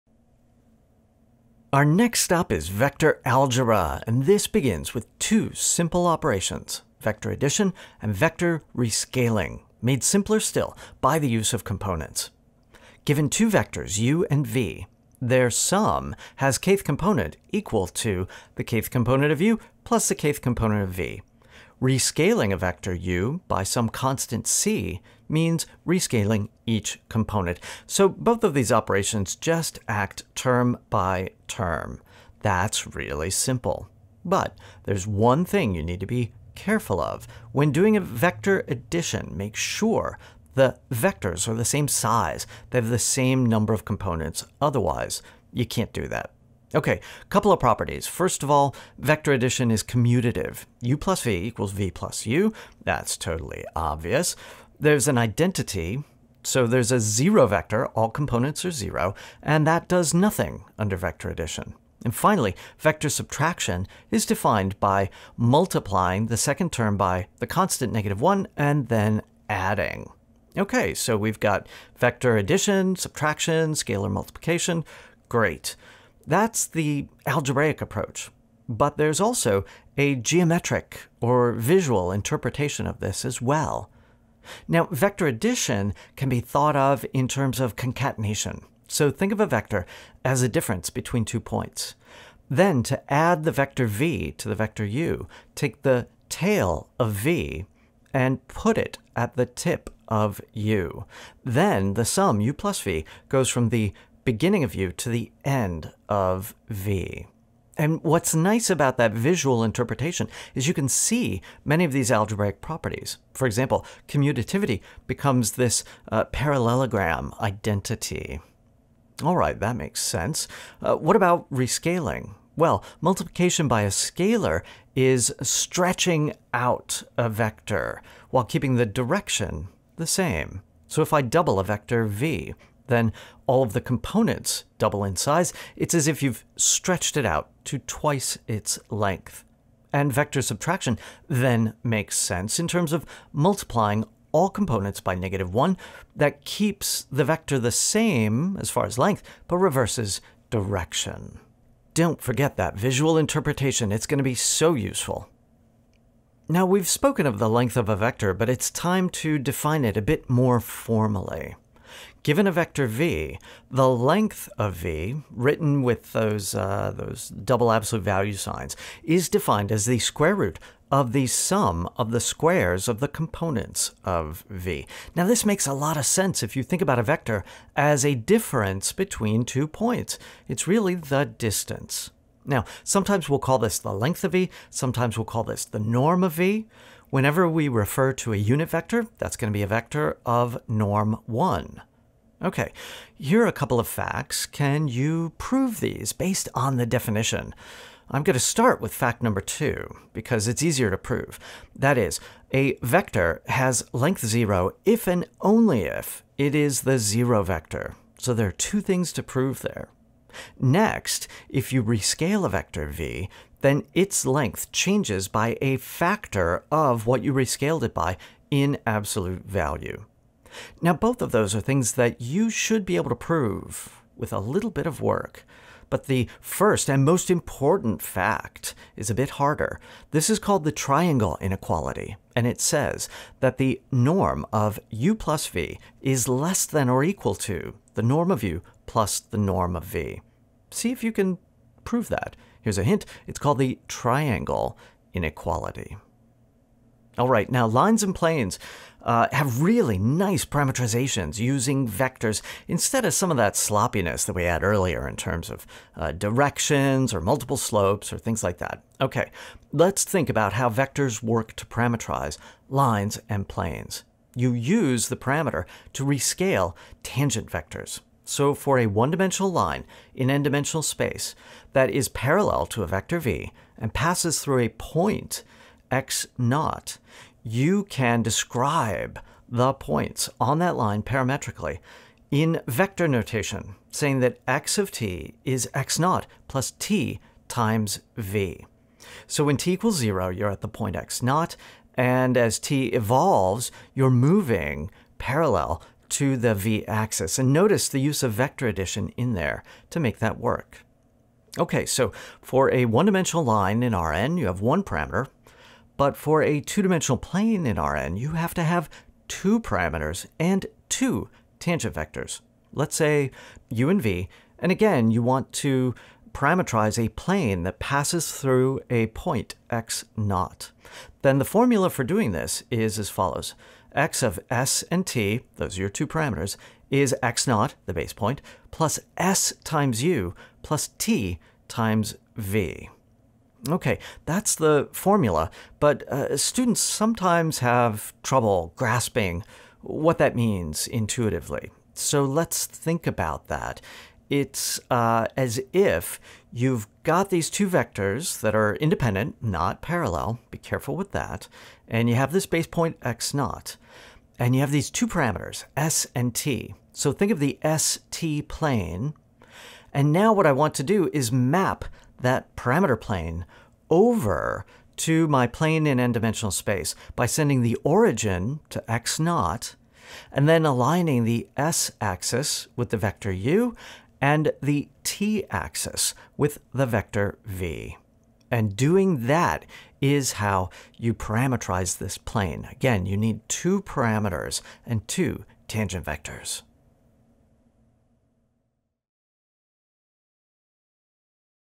CalcBLUE 1 : Ch. 4.2 : Basic Vector Algebra
What do we do with vectors? One source of utility comes from basic vector algebra. Let's consider how to add and rescale vectors.